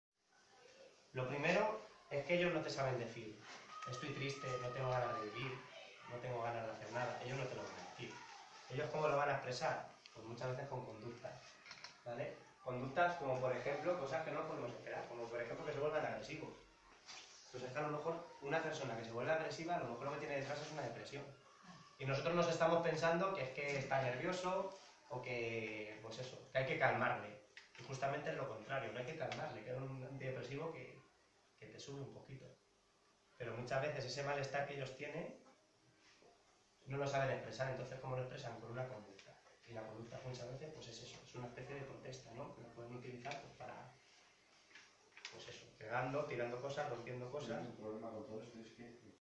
Afanias y formación
Exposición del psicólogo Mario García sobre problemas de conducta y salud mental en personas con discapacidad intelectual